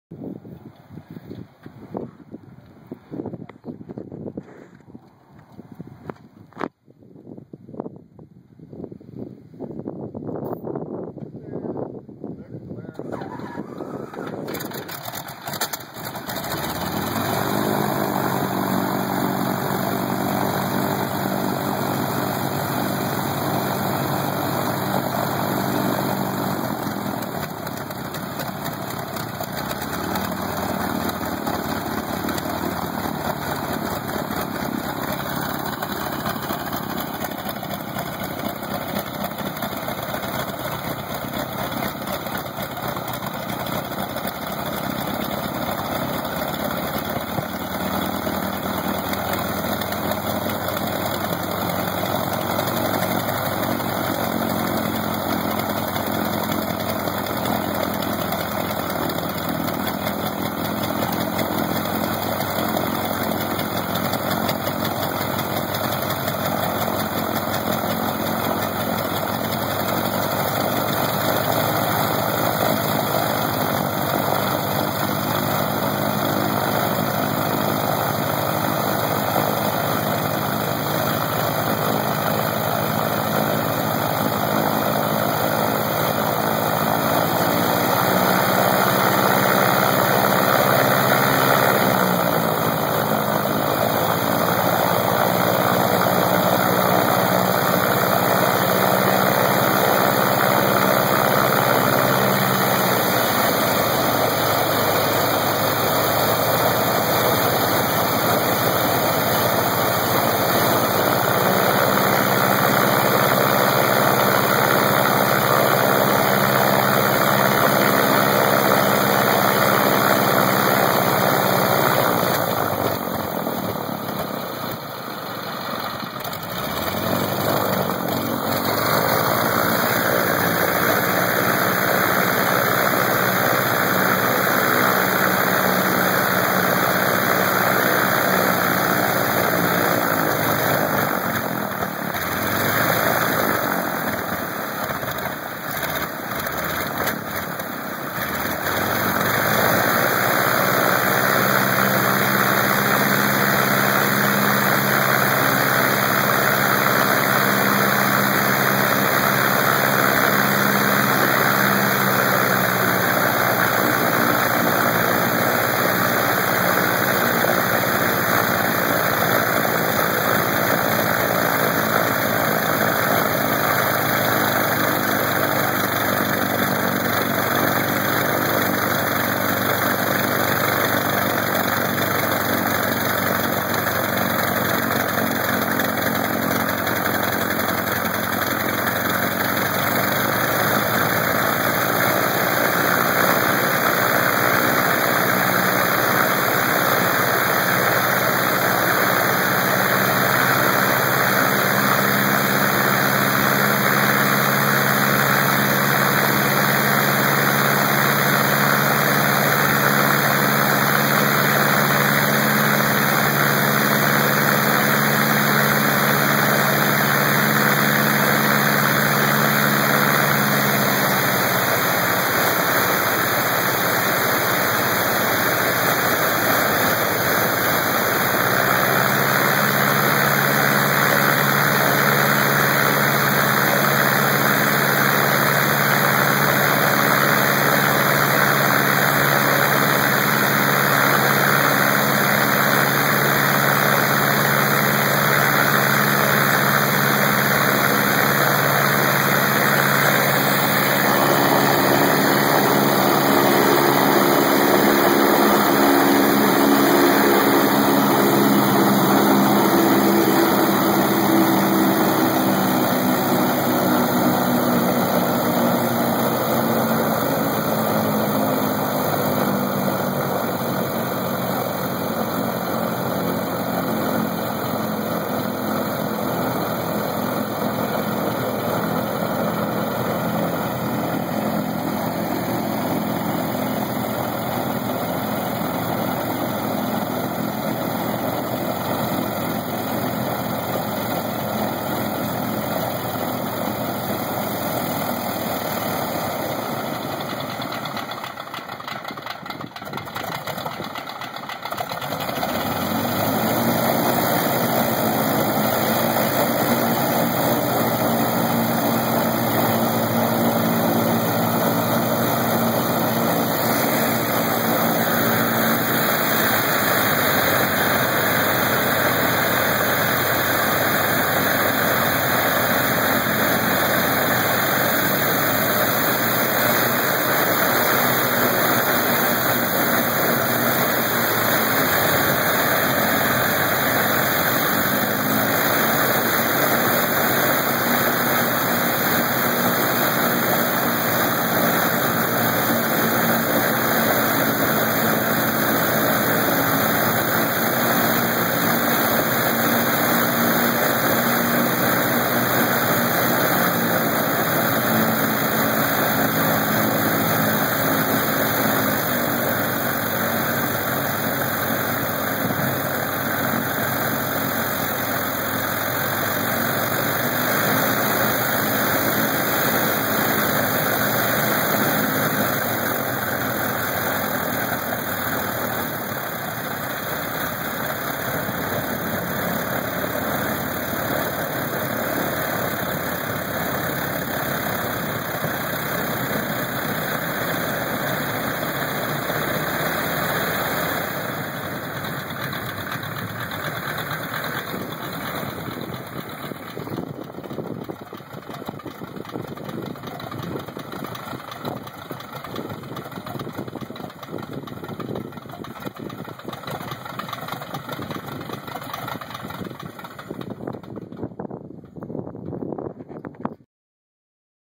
Lycoming R-680 Love The Scream!
After a decade of inaction, and several years outdoors, the old girl comes to life!  Lane Community College Aviation Maintenance student Eric Turk, along with former instructor Phil Foster and current Powerplant instructor Herb Moore, removed the carburetor and gave it a good cleaning.  She still has problems with the float, but that didn't stop the fellas from putting the spurs to this pretty old thing.

Love that roar!